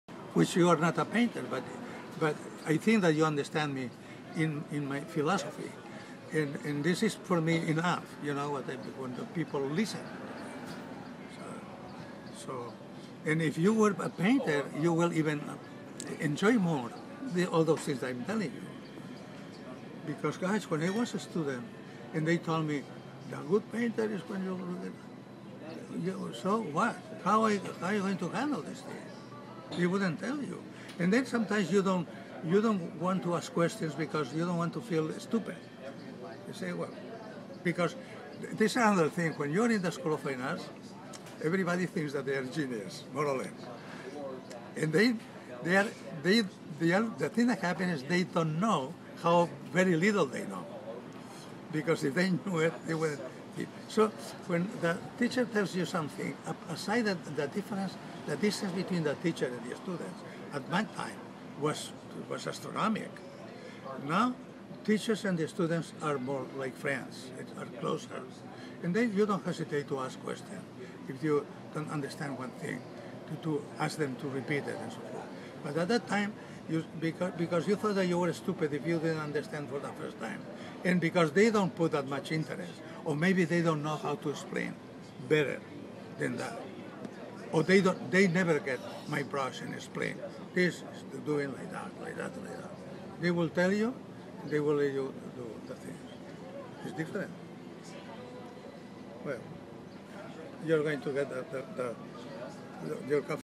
Sebastian Capella art conversation with Gary Gevisser - Ask and Listen
At Barbarella restaurant, La Jolla, California, June 22, 2011